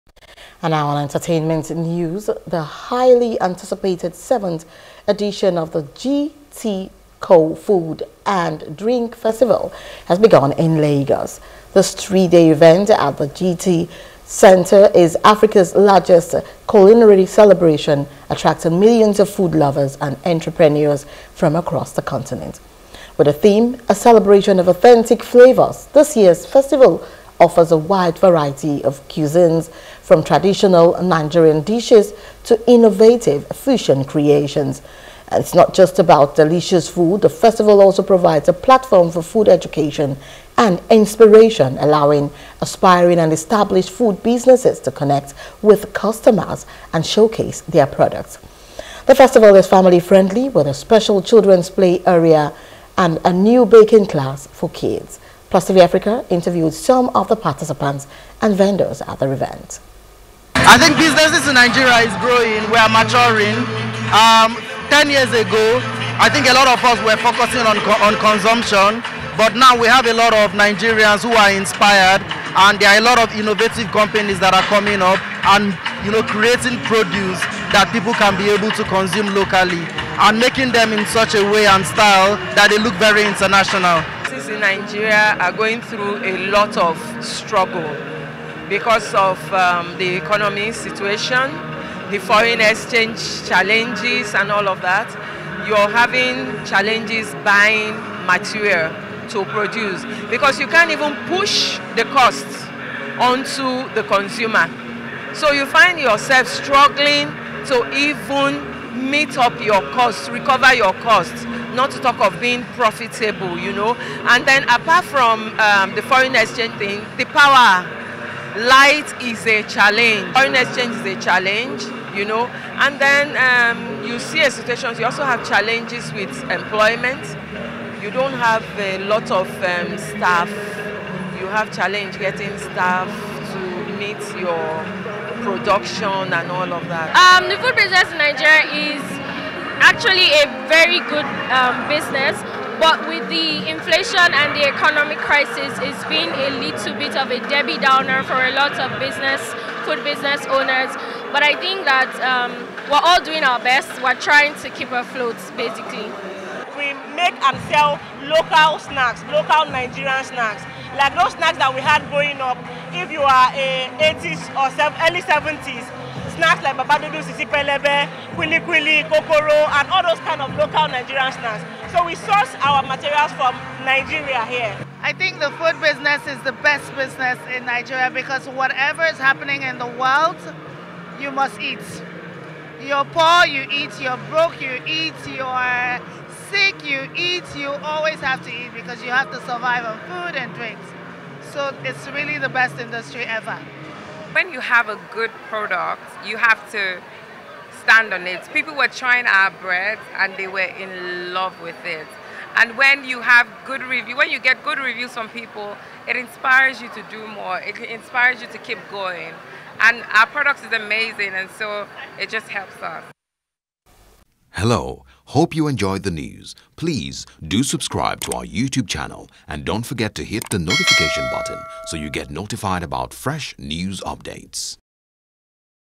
GTCO FOOD & DRINK: Event Showcases Diverse Culinary Heritage Of Africa
The highly anticipated 7th edition of the GTCO Food and Drink Festival has begun in Lagos.
This three-day event at the GTCentre is Africa's largest culinary celebration, attracting millions of food lovers and entrepreneurs from across the continent.
With the theme "A Celebration of Authentic Flavors," this year's festival offers a wide variety of cuisines, from traditional Nigerian dishes to innovative fusion creations.
 It's not just about delicious food; the festival also provides a platform for food education and inspiration, allowing aspiring and established food businesses to connect with customers and showcase their products.
The festival is family-friendly, with a special children's play area and a new baking class for kids.
Plus TV Africa interviewed some of the participants and vendors at the event.